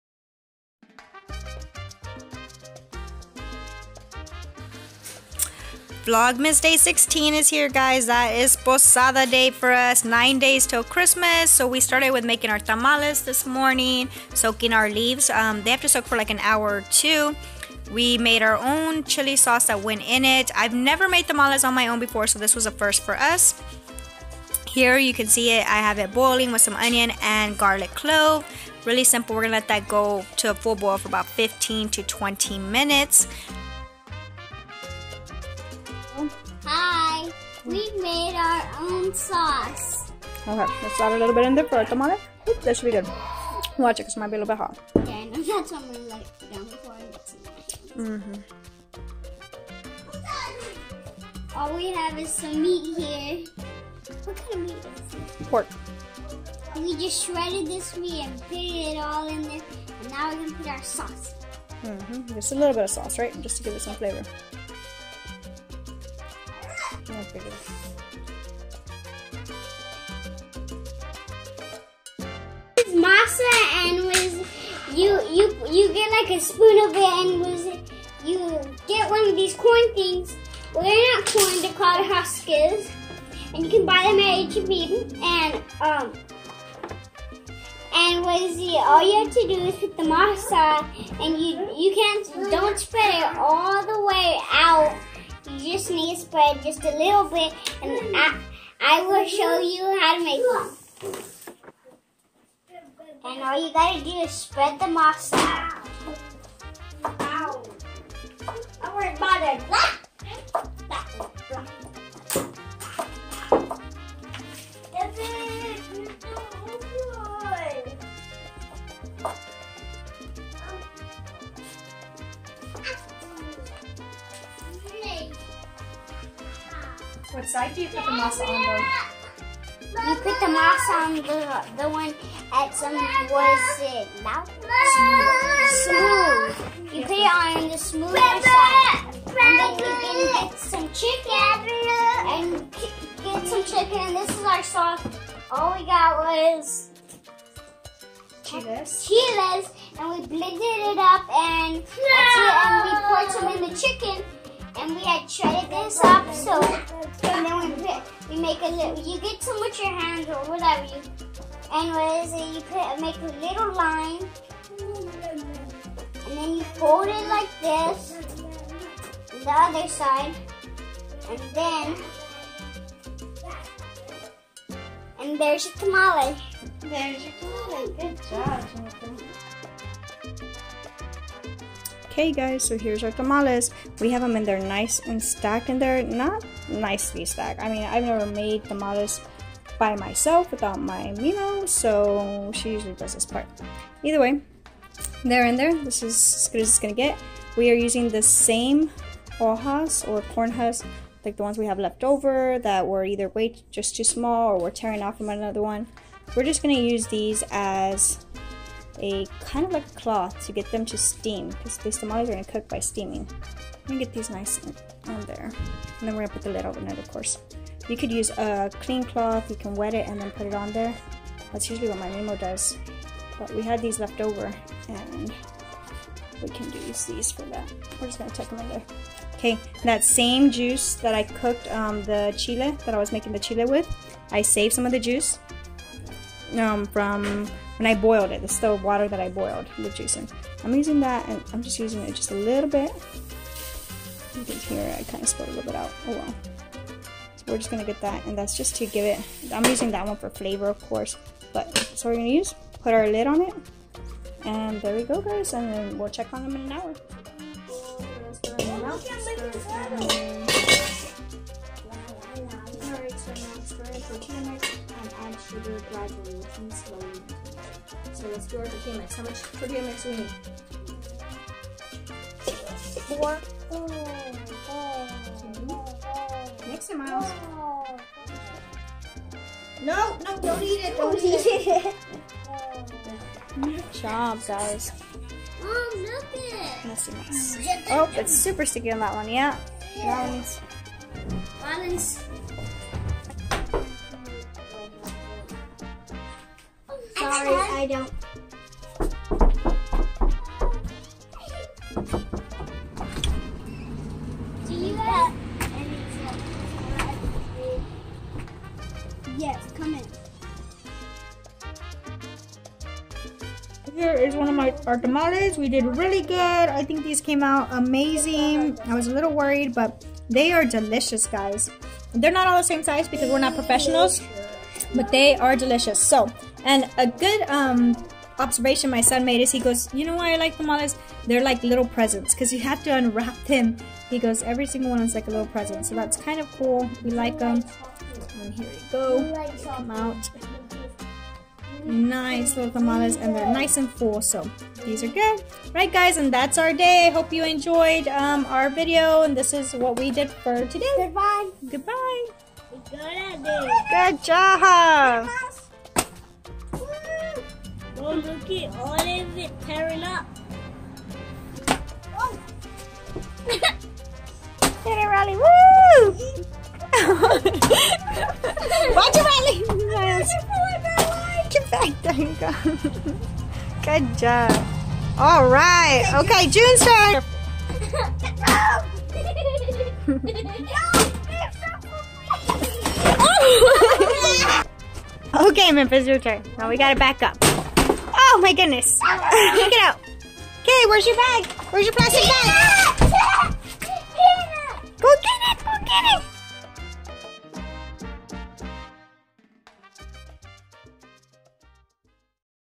Las Posada ||Tamales|| Vlogmas Day 16
Its the first day of Las Posada. In Mexico they celebrate with the 9 days till Christmas(Jesus was born), on the journey Mary & Joseph took during that time to Bethlehem.